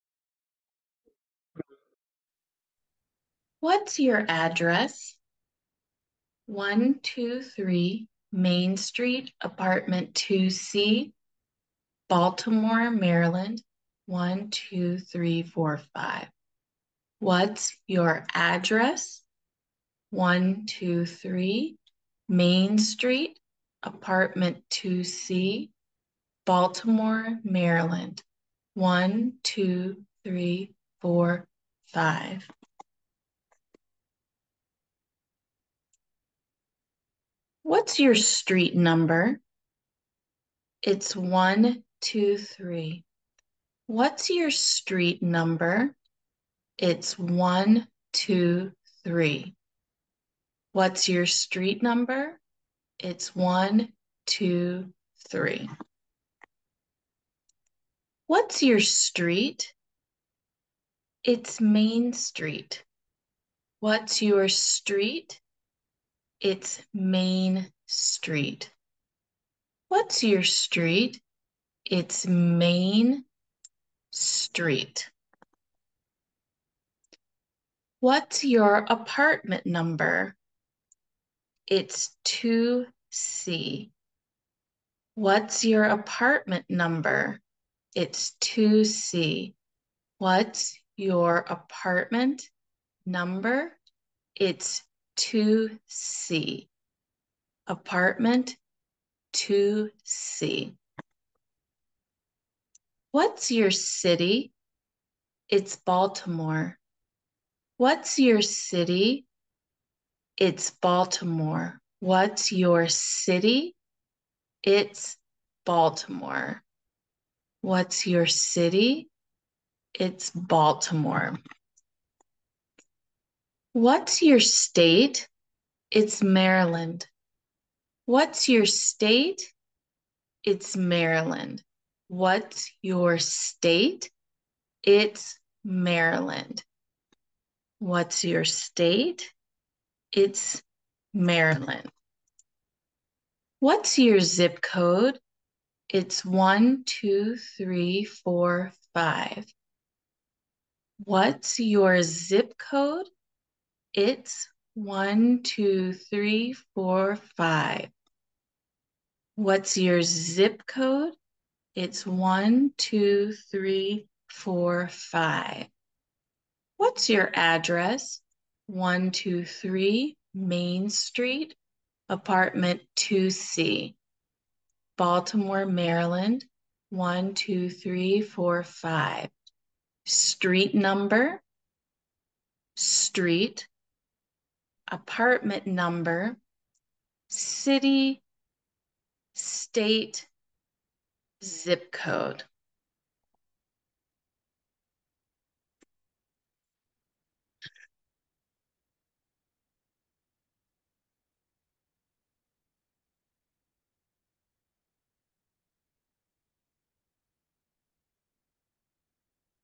What is your address? (Literacy and Beginner ESL Adult English Personal Information Practice)
Practice address reading and speaking fluency in this adult ESOL lesson with questions and answers/responses. (ESOL, ESL, ELL, EFL, English Language Learner) Also, practice pronunciation and vocabulary with color coding that makes the matching vocabulary appear naturally to new language learners.

Vocabulary: street, apt, number, city, state, zip code

Sample Slide: What’s your address? 123 Main Street Apartment 2C
What’s your street number? It’s 123.
What’s your street? It’s Main Street.
What’s your apartment number? It’s 2C.
What’s your city? It’s Baltimore.
What’s your state? It’s Maryland.
What’s your zip code? It’s 12345.

This vocabulary is used on
* U.S. Citizenship and Naturalization Interview questions and responses.
* High-frequency words for CASAS adult reading and listening tests and classrooms.
* Workforce and Development vocabulary for literacy and beginner adult English learners.
* Supplementing Future Intro Unit 7.
* Supplementing Stand Out Unit 1 Lesson 4.

Perfect for use in adult ESOL classrooms and lessons.